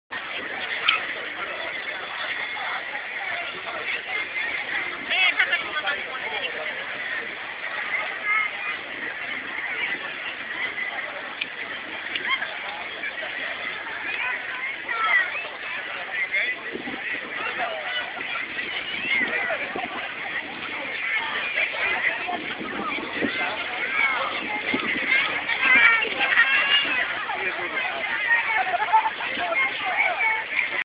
桜祭りを前に満開の小金井公園：風に揺れる桜花１
tube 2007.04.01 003 - koganei park cherry blossom 03